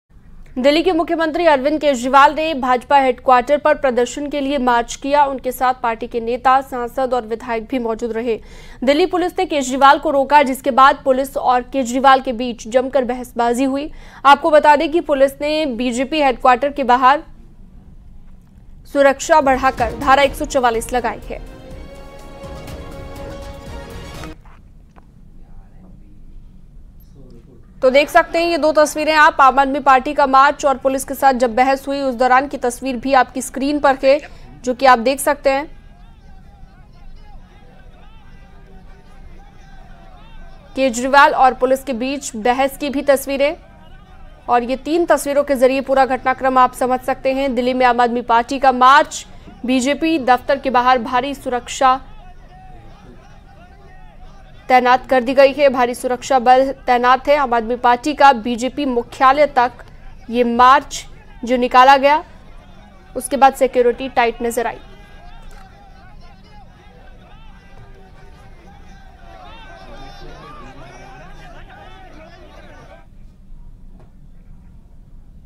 AAP Protest : केजरीवाल और पुलिस के बीच बहसबाजी | Arvind Kejriwal | Top News | Latest News | News18
भाजपा मुख्यालय की ओर आम आदमी पार्टी ने मार्च शुरू किया है। भाजपा मुख्यालय के बाहर भारी पुलिस बल तैनात किया गया है। वहीं इस दौरान पुलिस ने केजरीवाल को रोका. जिसके बाद पुलिस और केजरीवाल के बीच जमकर बहसबाजी शुरू हो गई.